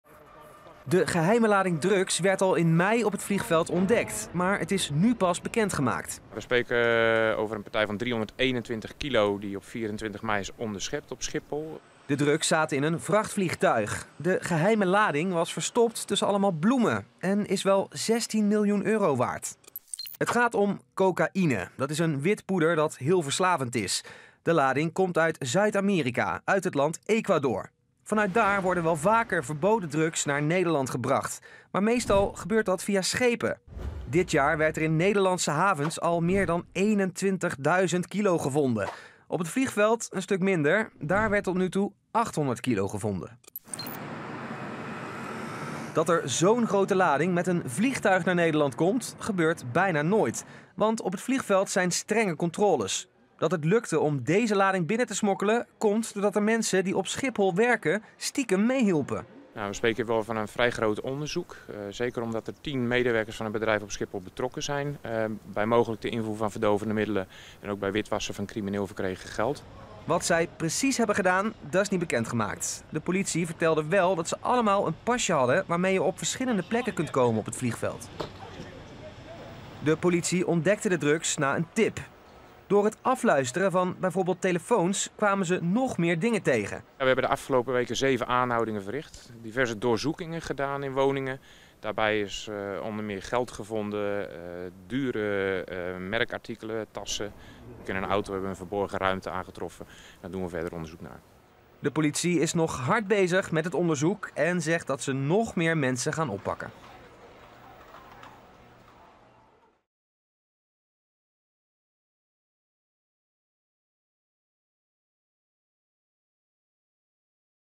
Enorme lading drugs gevonden op Schiphol
Door een geheime tip heeft de politie meer dan 300 kilo drugs gevonden op Schiphol. Zoiets is nog bijna nooit gebeurd op het vliegveld. De lading is miljoenen euro's waard.